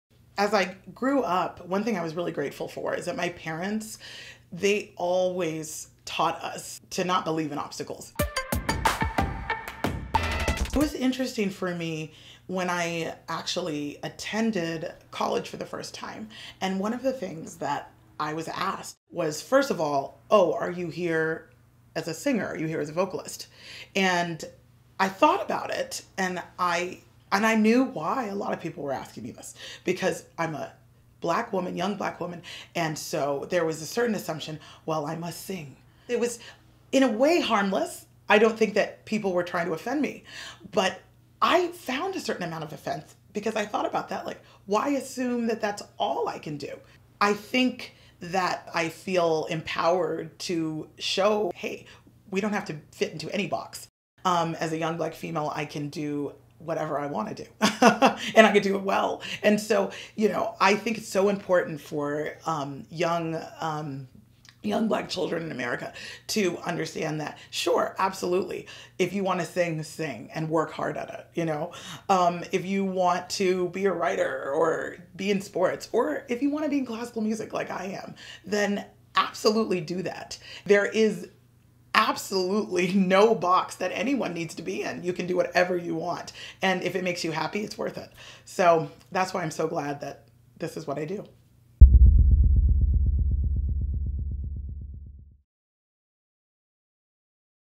How Michelle Cann breaks stereotypes in the world of classical music
Pianist Michelle Cann discusses the stereotypes she's faced in her career and how she's broken through them.